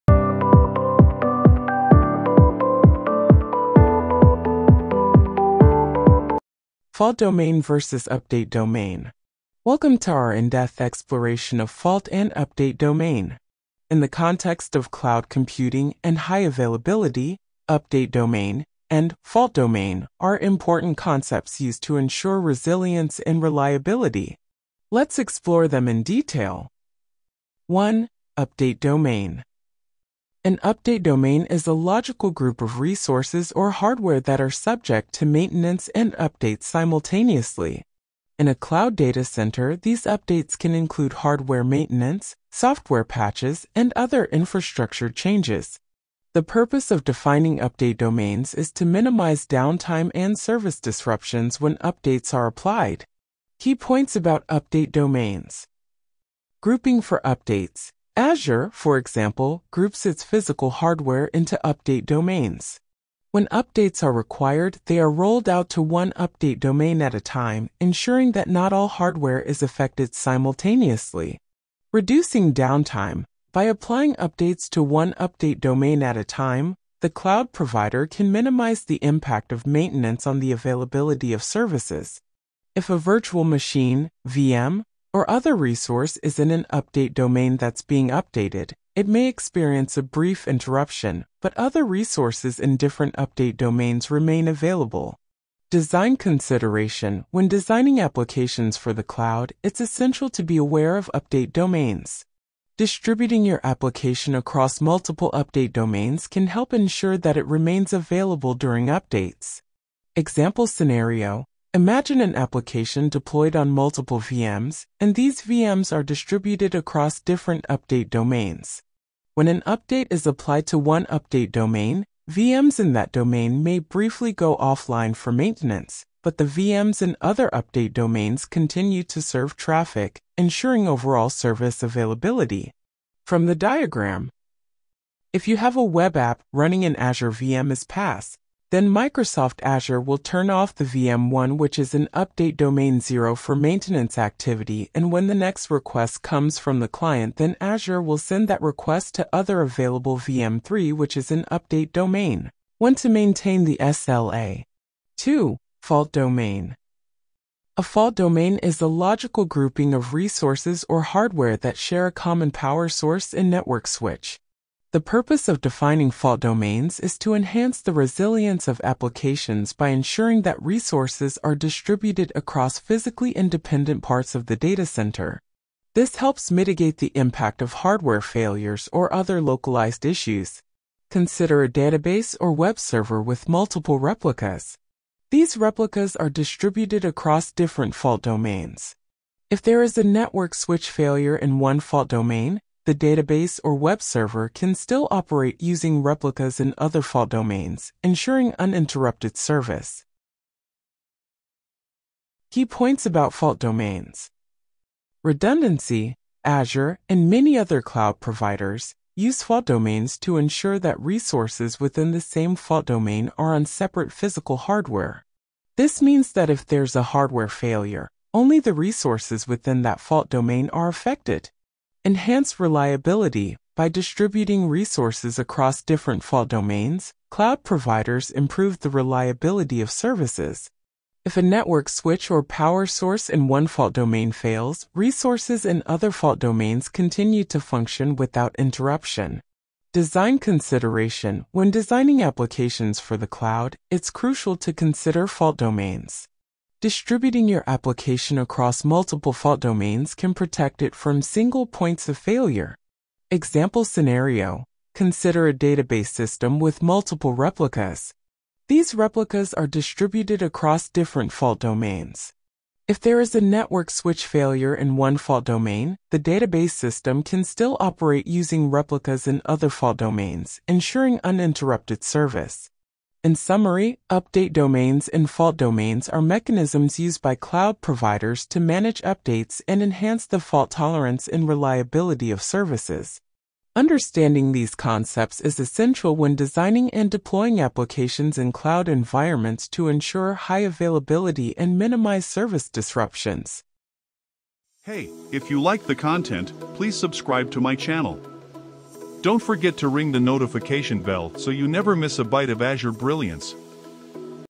what is azure update and fault domain
In this comprehensive video, we delve deep into the world of Azure Update and Fault Domains. Get ready to unlock the secrets behind these critical Azure concepts that are vital for ensuring high availability and reliability of your applications and services. From understanding their significance to practical tips and best practices, this video has you covered. Whether you're new to Azure or a seasoned pro, there's something here for everyone looking to master Azure's Update and Fault Domains.